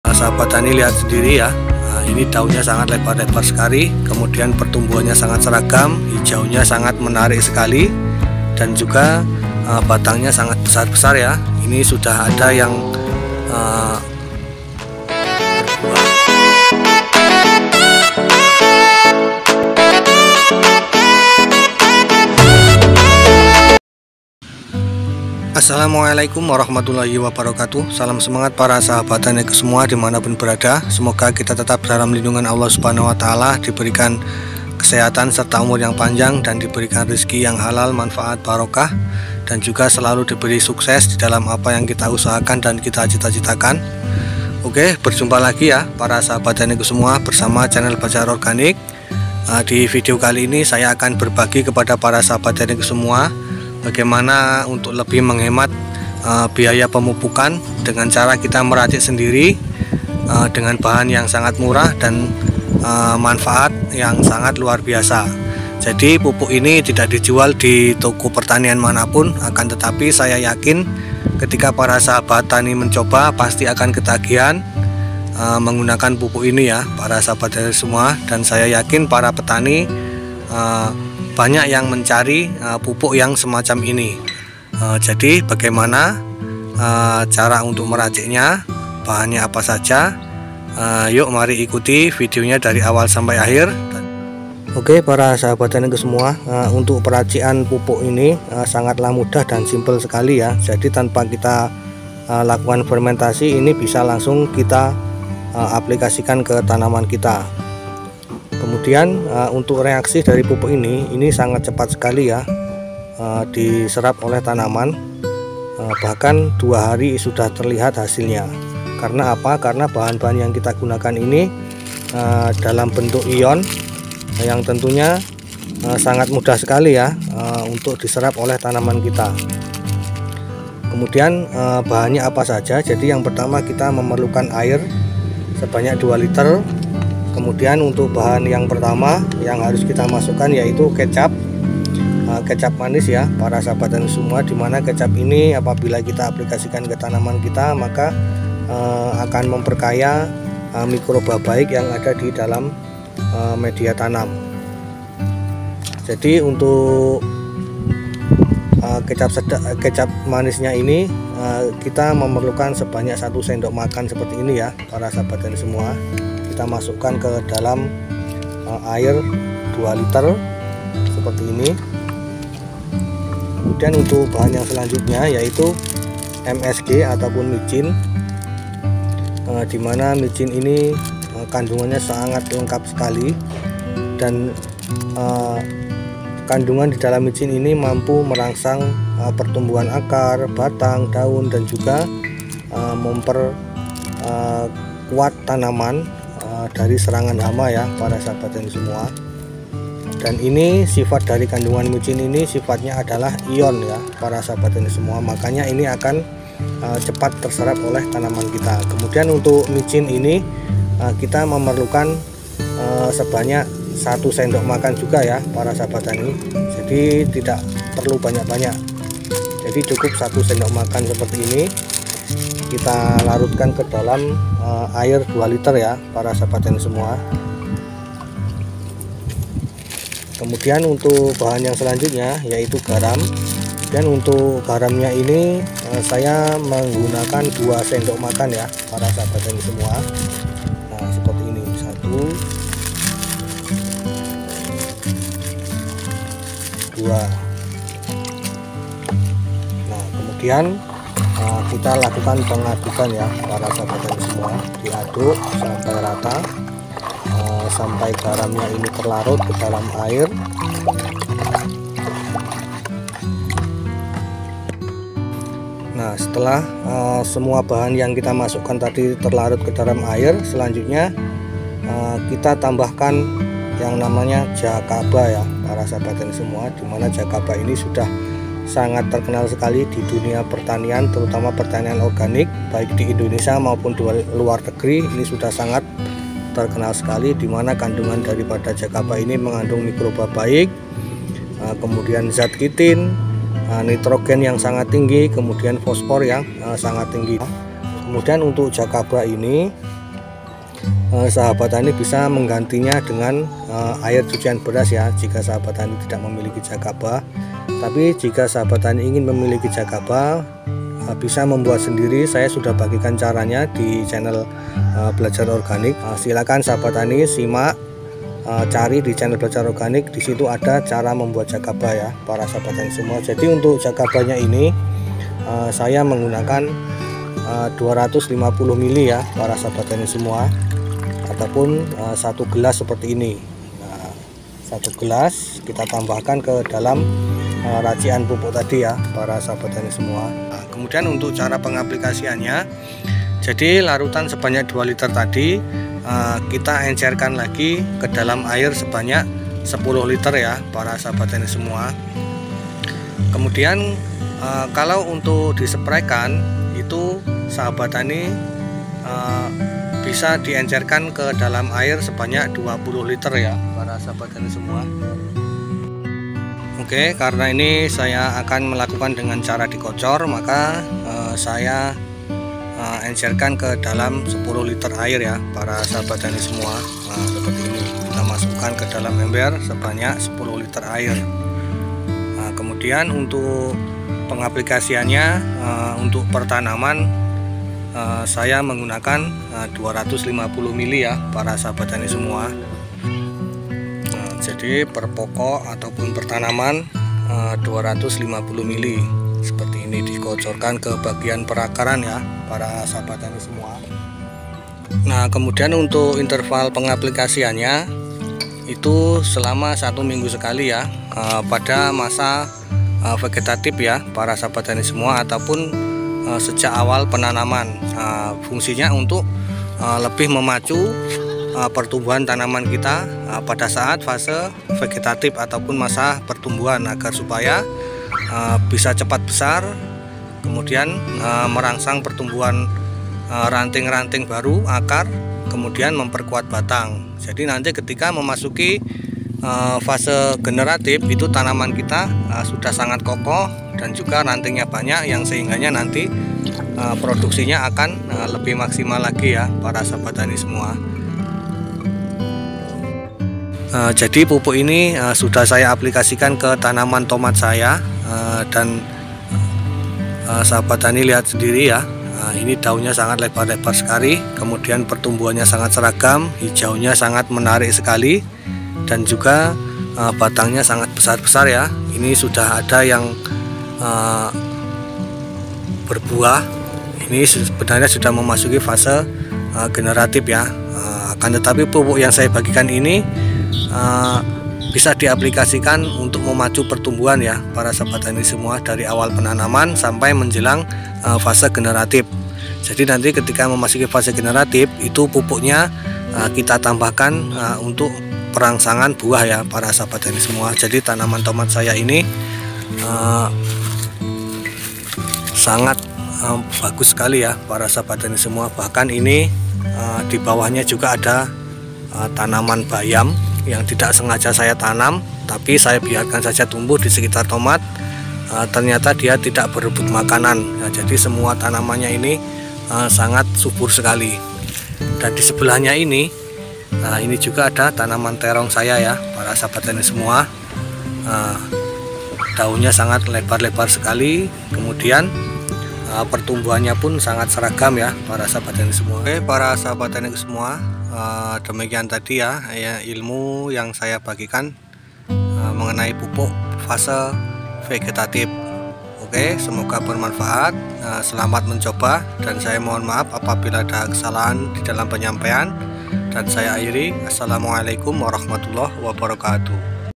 Pupuk pertumbuhan paling dicari jutaan petani
Saat tanaman berada pada vase vegetatif maka sangat membutuhkan nutrisi yang lengkap seperti nitrogen,pospor,kalium,kalsium, magnesium dan nutrisi penunjang lainnya agar mampu memaksimalkan pertumbuhan dan pembentukan bagian tanaman seperti akar,batang,daun tunas dan juga memperbanyak ranting,sehingga ketika tanaman masuk vase generatif maka tanaman sudah siap dan kokoh dan tentunya produksinya bisa maksimal dan lebih kebal dari serangan hama dan penyakit.
Maka pada vidio ini Chanel @88samsulorganik membagikan cara meracik pupuk sendiri yang fungsinya lebih banyak dan reaksinya cepat terserap oleh tanaman,dengan bahan yang murah mudah dalam pembuatan Tanpa Ribet tanpa fermentasi bisa langsung diaplikasikan pada tanaman.
Semoga vidio ini bisa memberi manfaat buat para sahabat taniku semua.